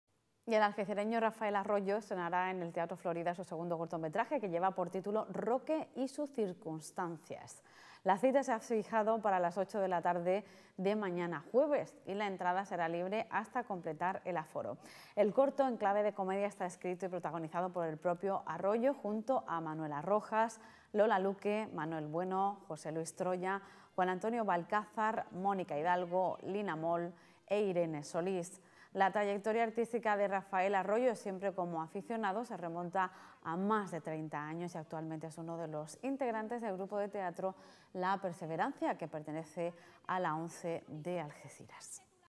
El algecireño Rafael Arroyo estrena mañana su segundo cortometraje en el Florida
El algecireño Rafael Arroyo estrenará en el Teatro Florida su segundo cortometraje, que lleva por título “Roque y...sus circunstancias”. La cita se ha fijado para las ocho de la tarde de mañana jueves , y la entrada será libre hasta completar el aforo. El corto, en clave de comedia, está escrito y protagonizado por el propio Arroyo junto a Manuela Rojas, Lola Luque Manuel Bueno, José Luis Troya, Juan Antonio Balcázar, Mónica G. Hidalgo, Lina Moll, e Irene Solís. La trayectoria artística de Rafael Arroyo, siempre como aficionado, se remonta a más de treinta años, y actualmente es uno de los integrantes del grupo de teatro “La perseverancia”, que pertenece a la ONCE de Algeciras.